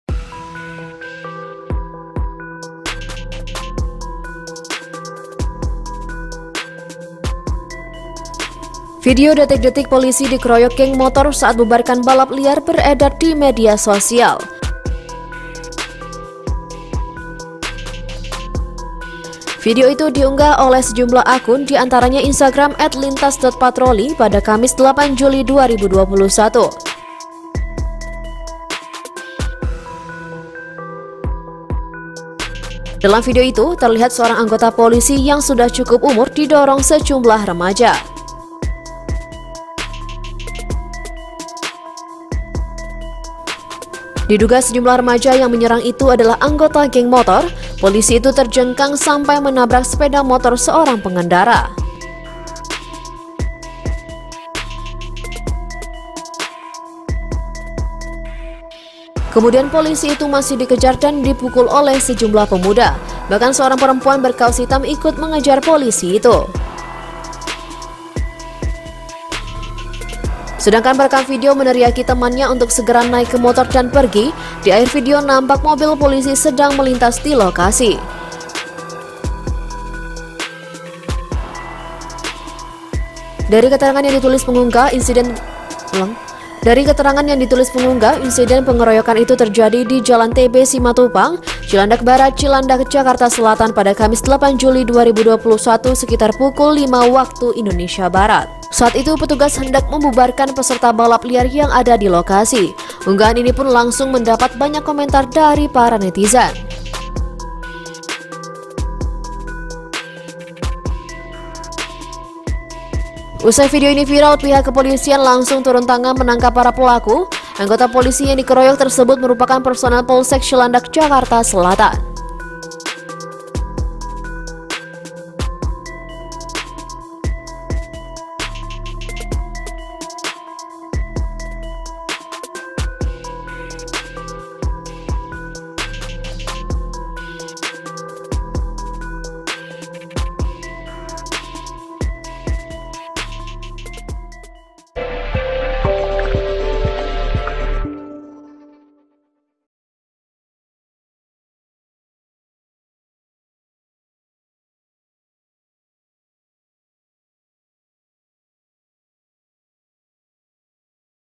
VIRAL POLISI DIKEROYOK GENG MOTOR SAAT BUBARKAN BALAP LIAR
Video itu diunggah oleh sejumlah akun, di antaranya Instagram @lintas.patroli pada Kamis (8/7/2021).

Dalam video itu terlihat seorang anggota polisi yang sudah cukup umur didorong sejumlah remaja.

Diduga sejumlah remaja yang menyerang itu adalah anggota geng motor.

Polisi itu terjengkang sampai menabrak sepeda motor seorang pengendara.

Kemudian polisi itu masih dikejar dan dipukul oleh sejumlah pemuda.

Bahkan seorang perempuan berkaos hitam ikut mengejar polisi itu.

Sedangkan perekam video meneriaki temannya untuk segera naik ke motor dan pergi.

Di akhir video nampak mobil polisi sedang melintas di lokasi.

Dari keterangan yang ditulis pengunggah, insiden pengeroyokan itu terjadi di Jalan TB Sumatupang, Cilandak Barat, Cilandak, Jakarta Selatan pada Kamis (8/7/2021) sekitar pukul 05.00 WIB.

Saat itu petugas hendak membubarkan peserta balap liar yang ada di lokasi.